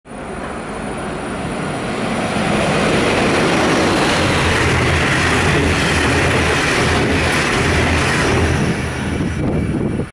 Így láthatja egy hangya a NYÍRSÉG IC-t
Mozdony: 480 014
Kocsik: Bpee, Bpee, Bpee, Bdbpmz, Apee,
Vonatszám: 605
A vonat Nyíregyháza - Budapest-Nyugati pu. viszonylatban közlekedett.
A videó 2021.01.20-án 12:47-kor készült Karcagon.